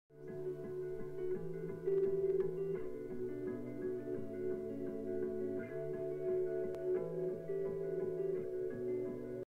fools lgbt+ warriors map part 10
part 10 for the fools lgbt+ warriors 24 hour map the characters i chose are hollyleaf and ivypool